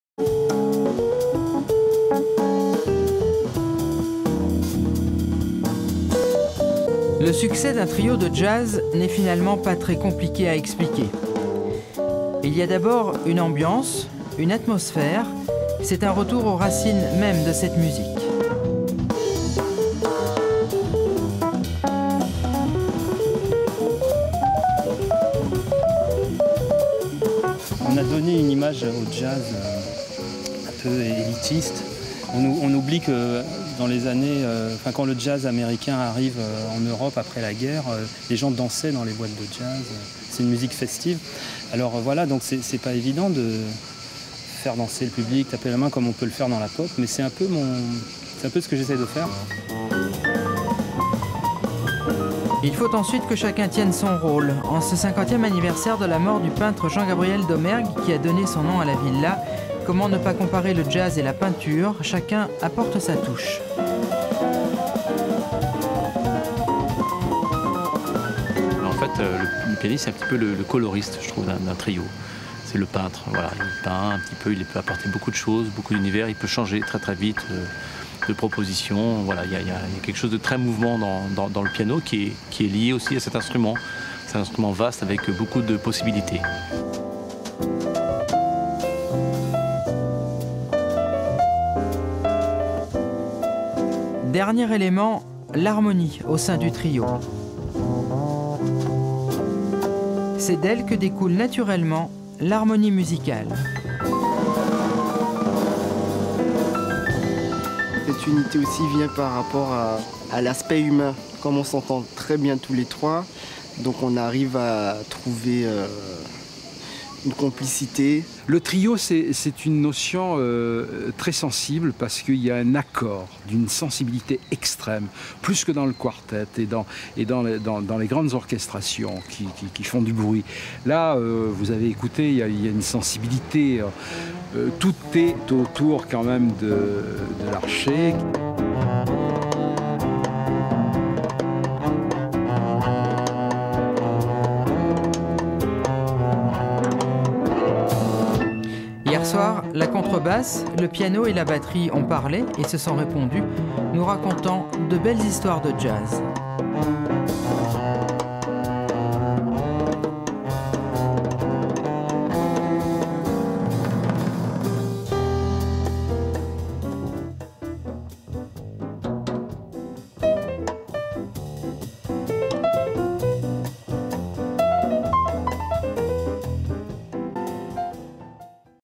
Festival "Jazz à Domergue" sur les hauteurs de Cannes (06)
Contrebasse, piano, batterie, ce sont les instruments du trio Laurent Larchet qui se produisait hier à jazz à Domergue, sur les hauteurs de Cannes. Les instruments incontournables pour jouer des notes bleues.
Plus d'infos sur france3cotedazur.fr